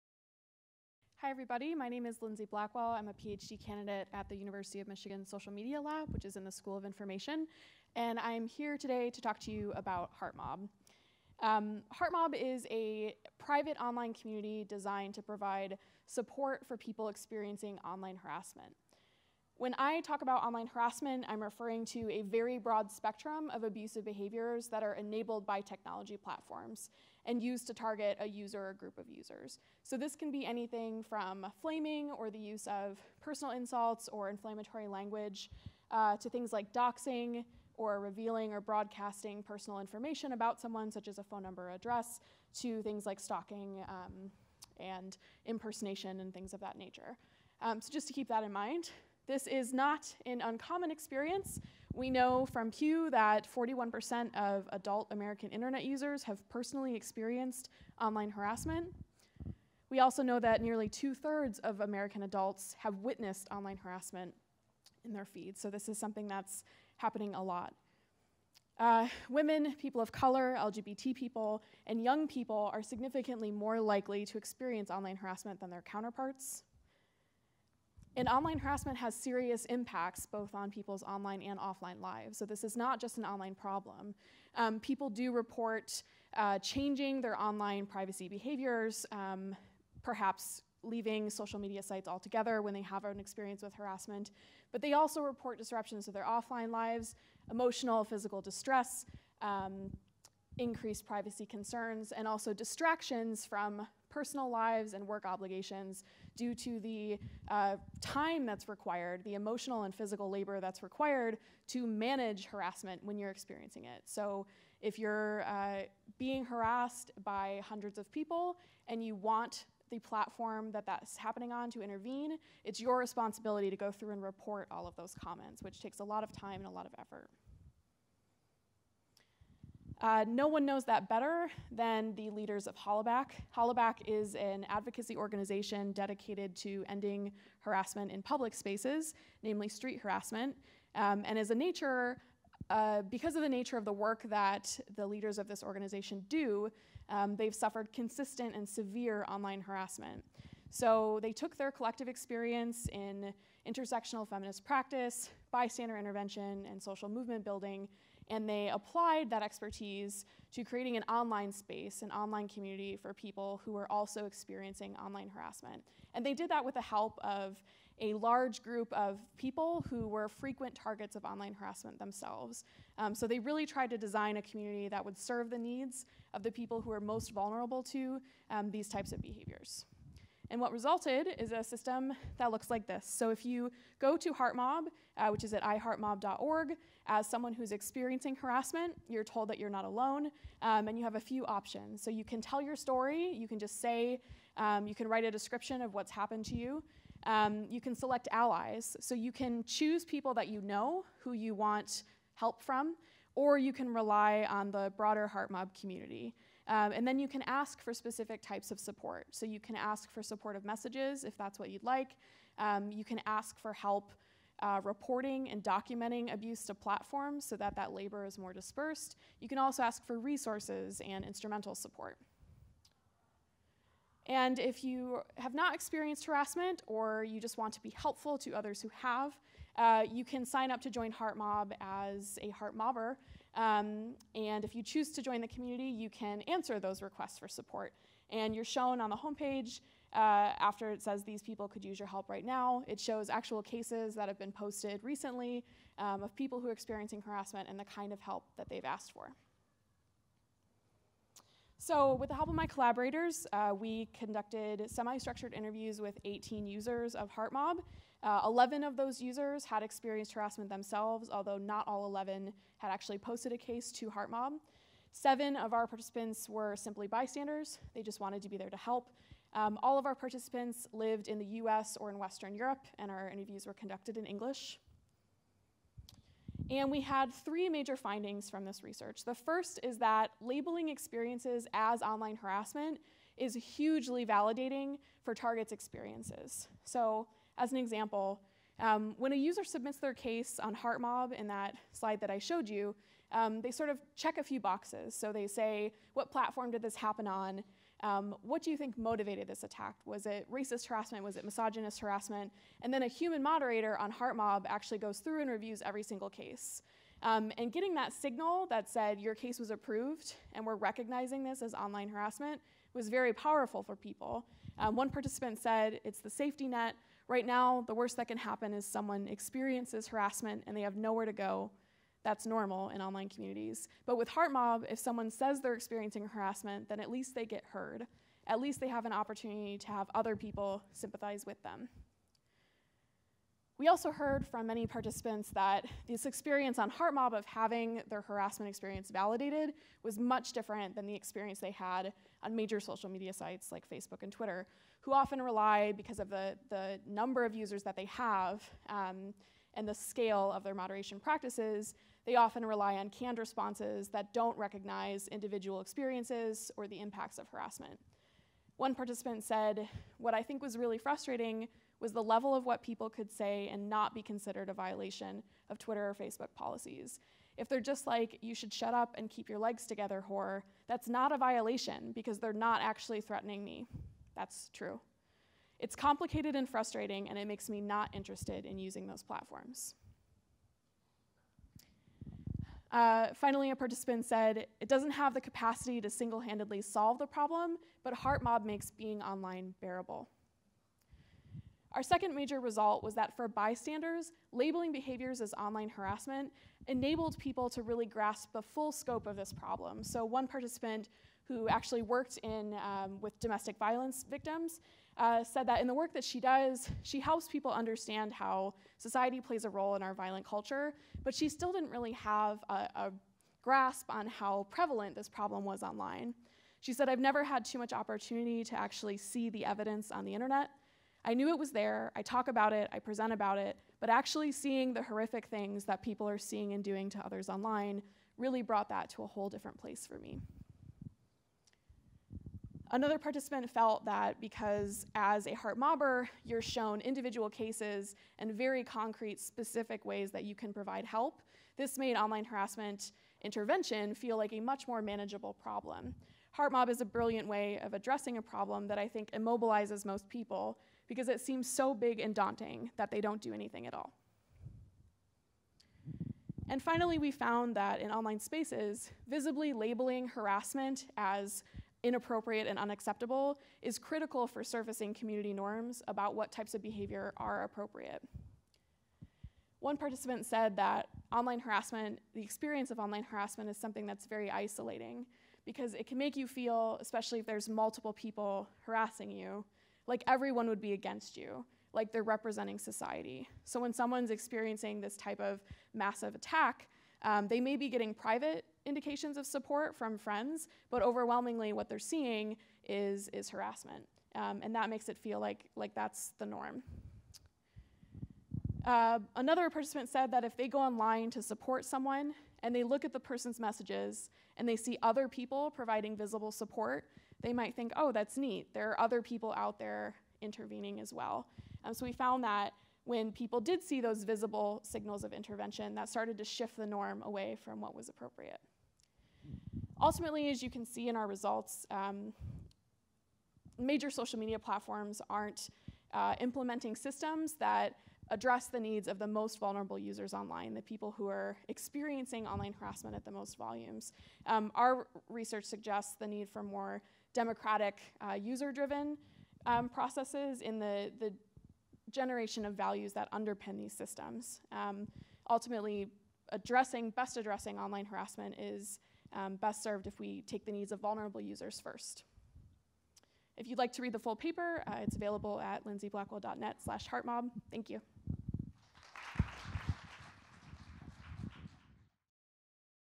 HeartMob: Community Support for Online Harassment Targets: Lindsay Blackwell at CSMIT2018
Lindsay Blackwell discusses her research on Heartmob, an online community where targets of online harassment can report their experiences and seek allies, and describes the positive impact Heartmob has on its users.

Lindsay was speaking at the CivilServant Community Research Summit at MIT in January 2018.